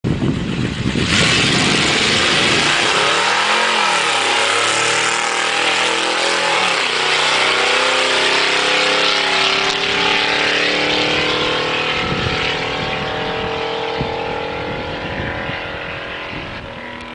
2009 SDC Int'l Meet Drags Jeff Rice Video 1
Unnofficial drags held near Cedar Rapids, Iowa